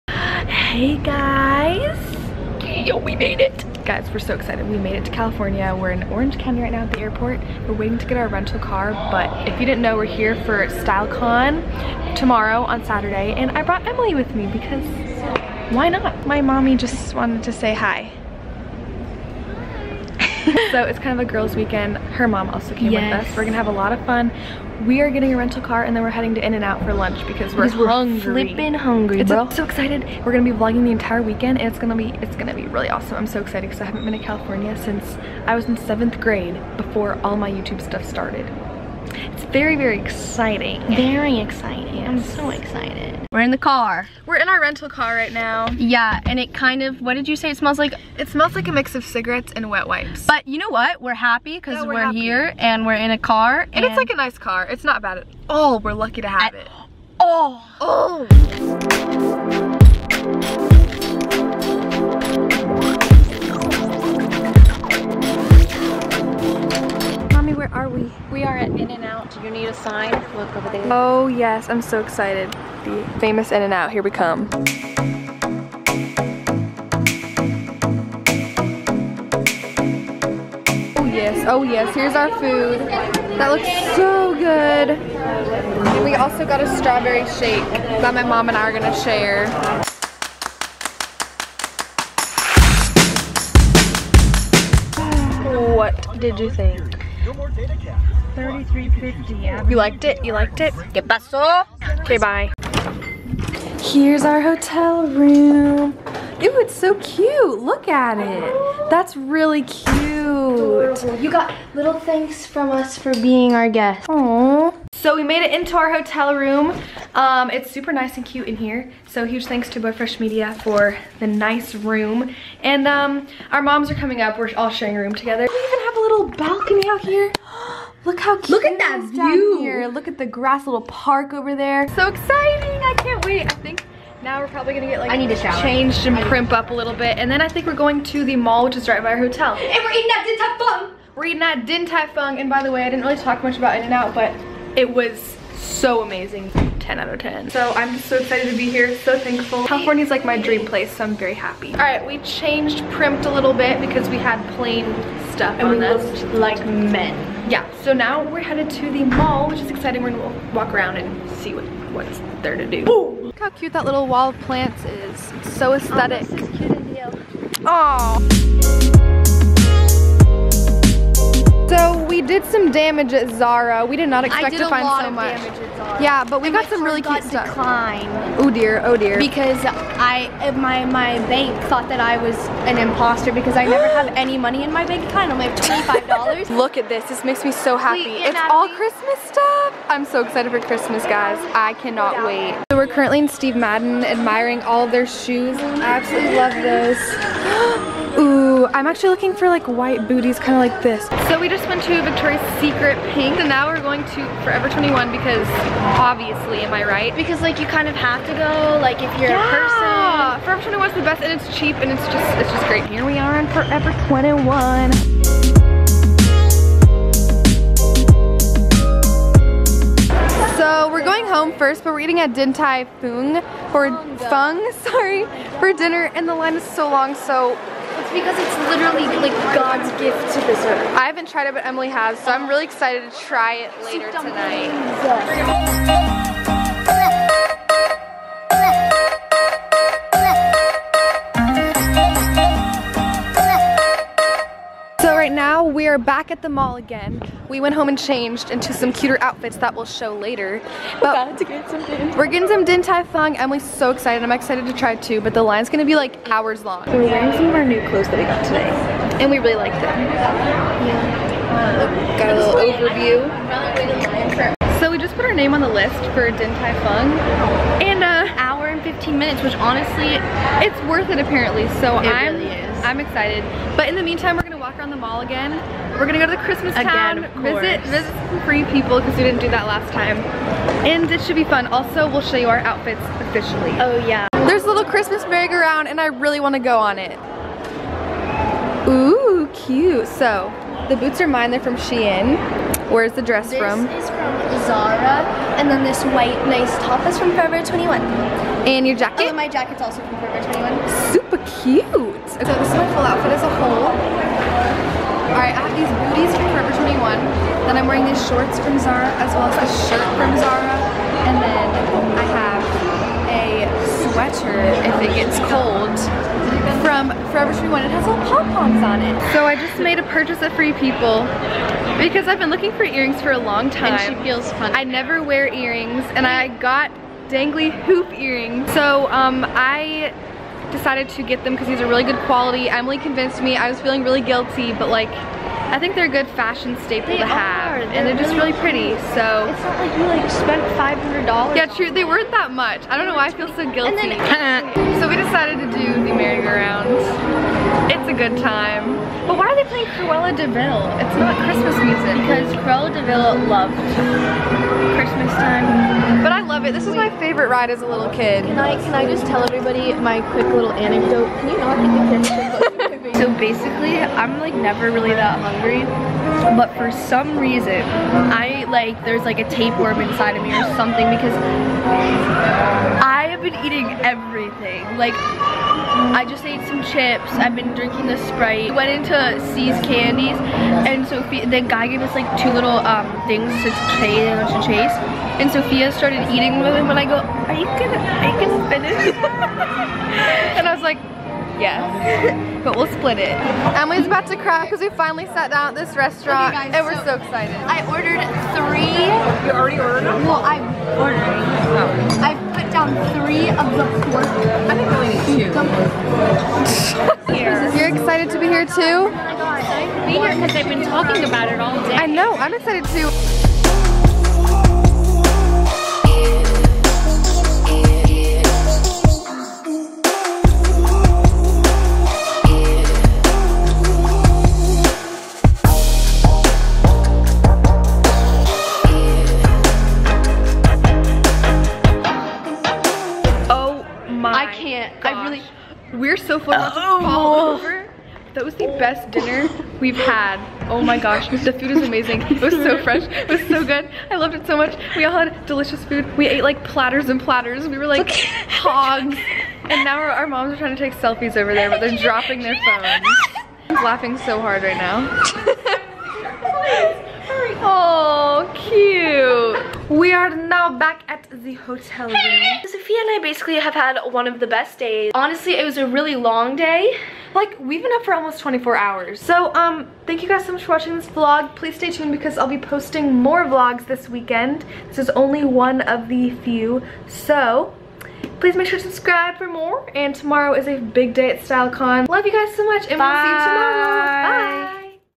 Girls' Trip in California!! | Food, Shopping + Fun!!
Woohoo for another vloggggg!! Today, we arrivedin Orange County, CA!! I'm so gratefu for the fun day we had and I can't wait for you guys to see more of our weekend. Thank you guys for watching 💗